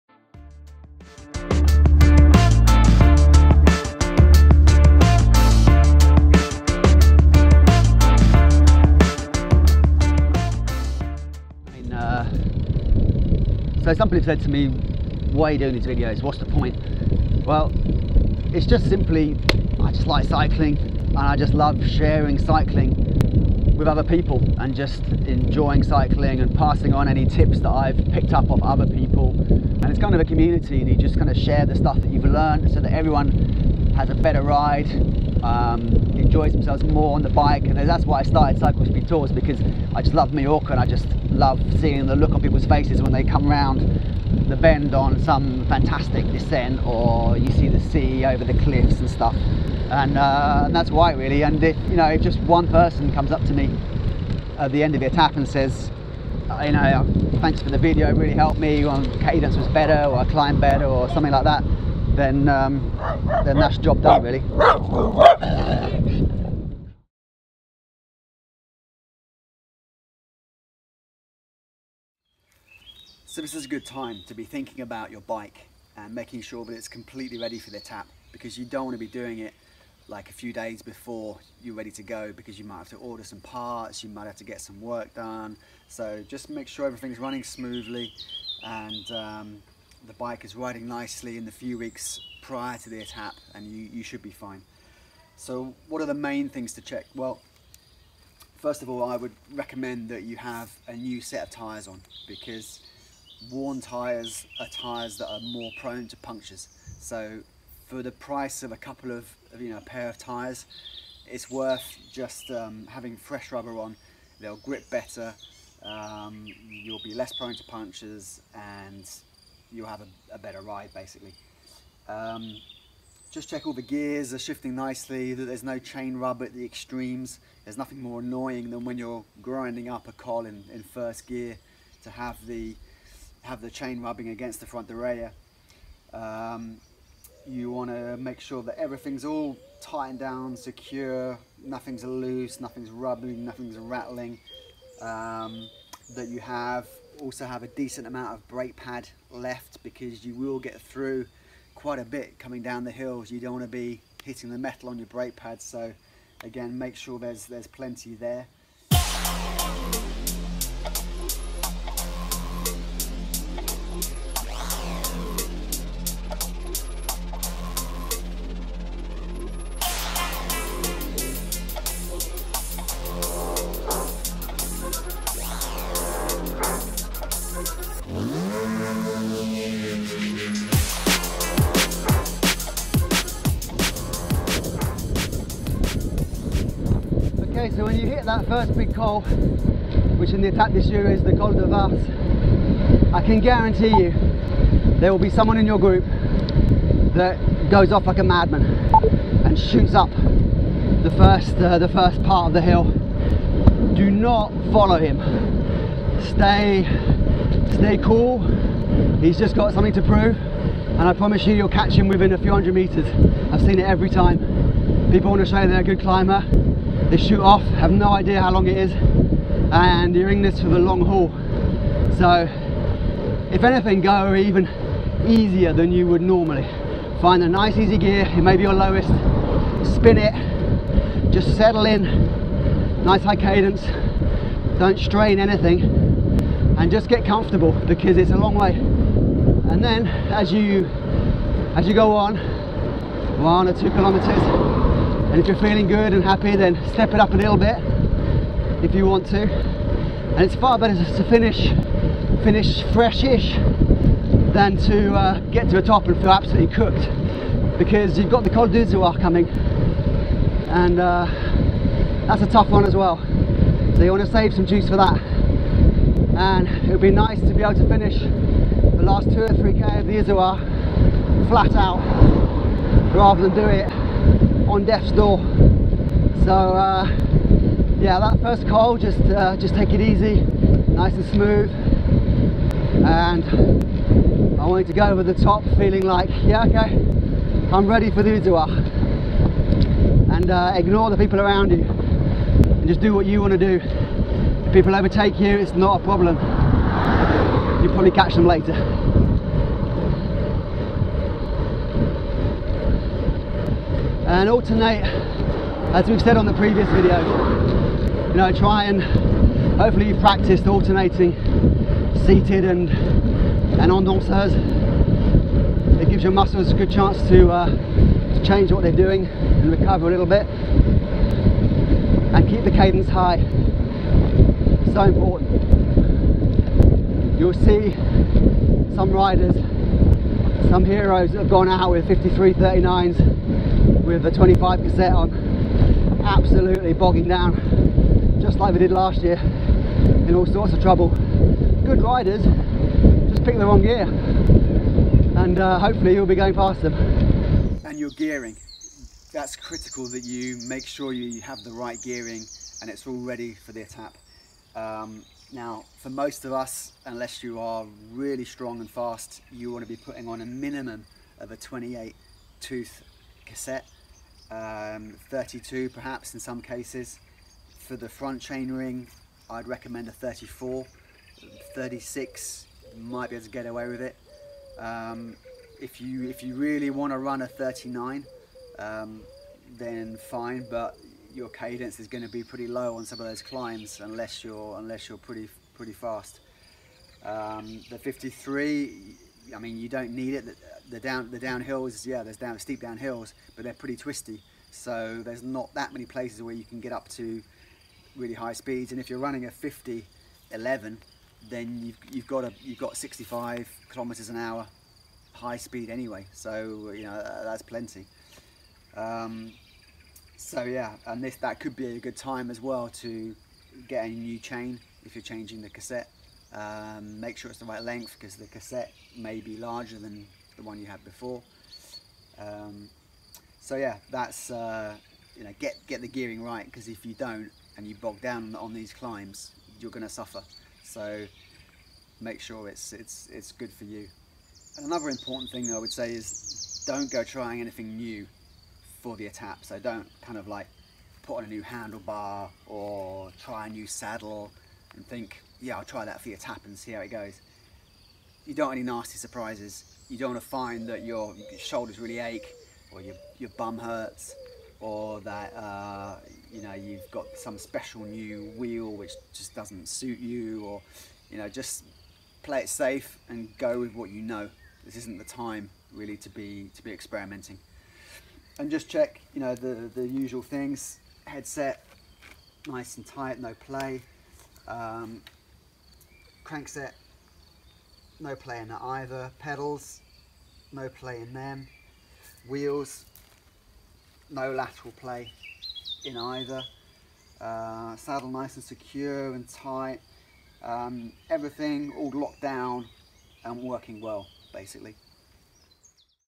Pacing Your Climb, Preparing your Bike : Etape du Tour and Marmotte 2017; June -
The Penultimate Episode looks at how to pace your climbs and how to prepare your bike for the BIG DAY!